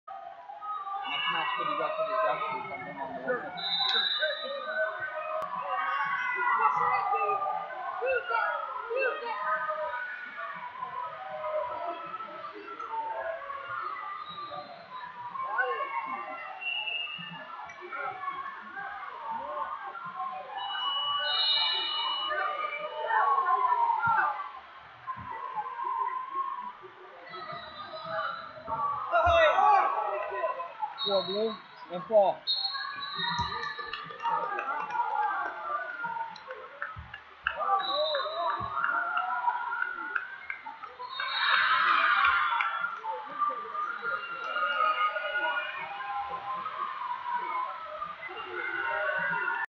Mat 15 106 Davis Bourgeois Louisiana Vs Noah Savageau North Dakota Blue
2019 Junior Duals - Mat 15 106 Davis Bourgeois Louisiana Vs Noah Savageau North Dakota Blue